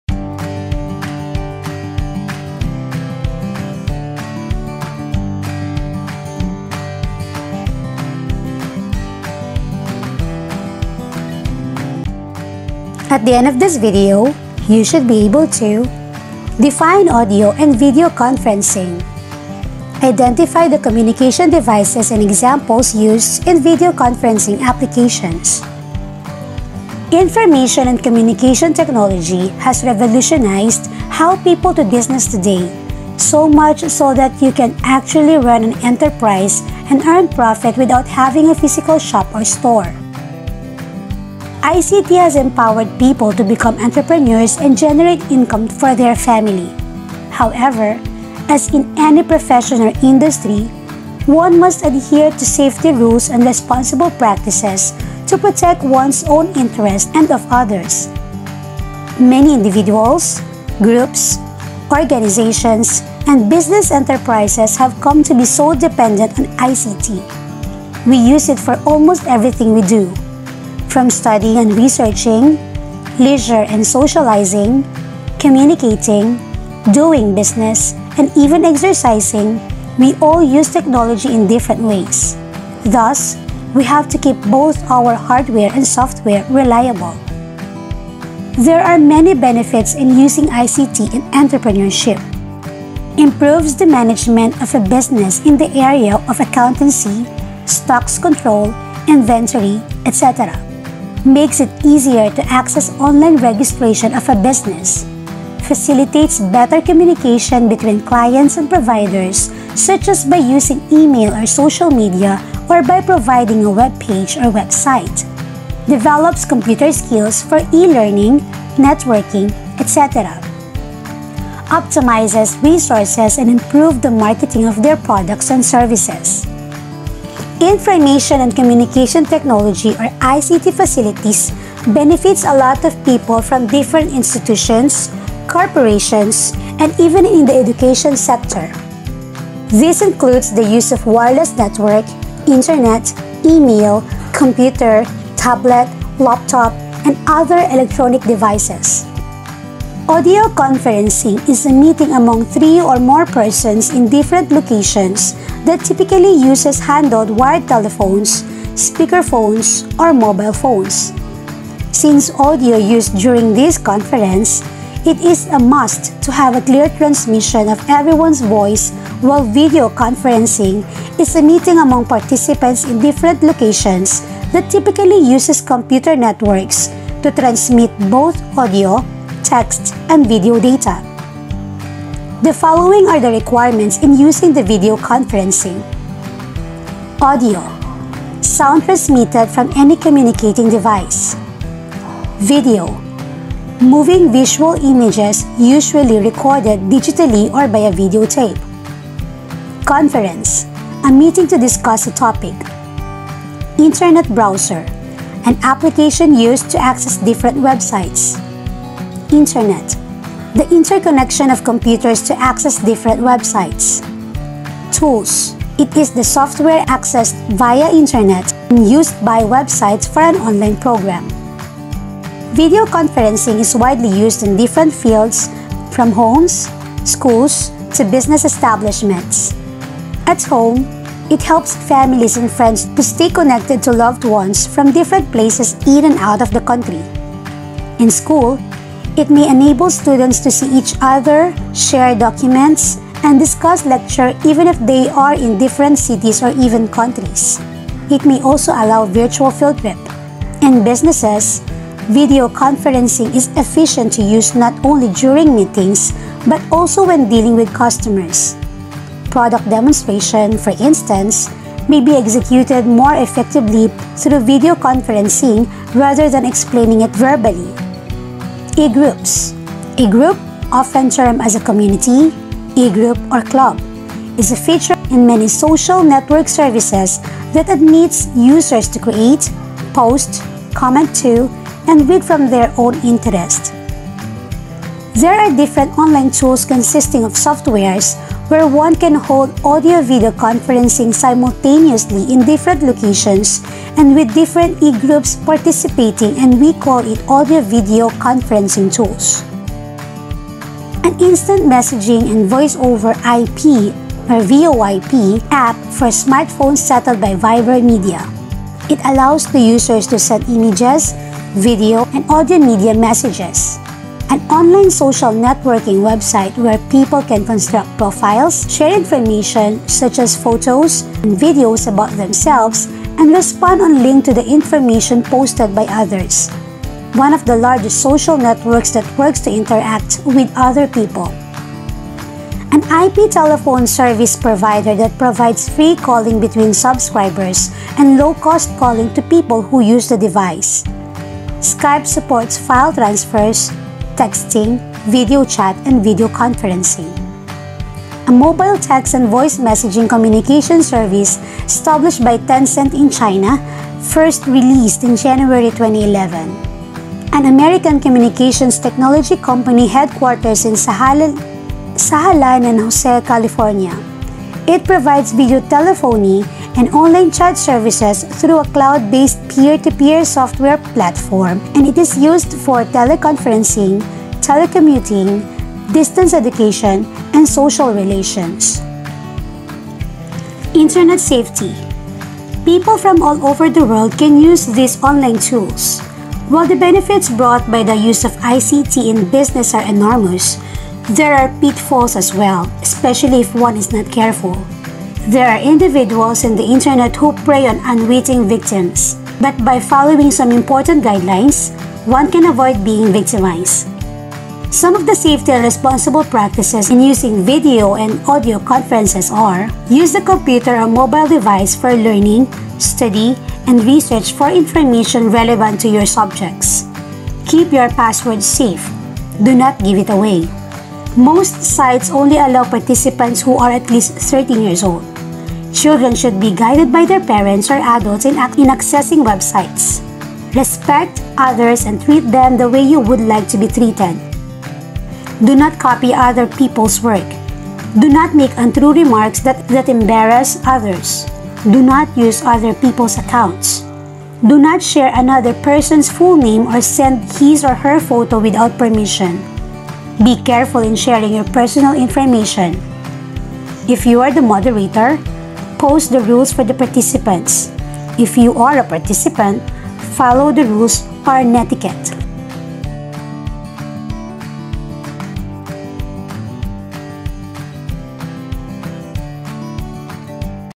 TLE 6 QUARTER 3 WEEK 4
SAFE AND RESPONSIBLE USE OF AUDIO AND VIDEO CONFERENCE

1. Define audio and video conferencing
2. Identify the communication devices and examples used in video
    conferencing applications